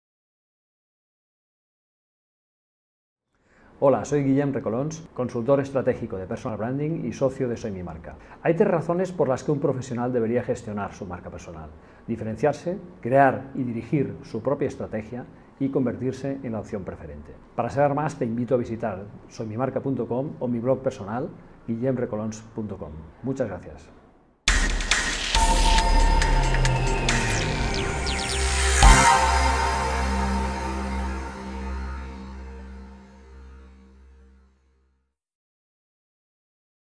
Intro Guillem Recolons
Breve presentación en formato microstim video de Guillem Recolons, una videortarjeta de visita que permite su envío en mensajería instantánea (Whatsapp, Line...)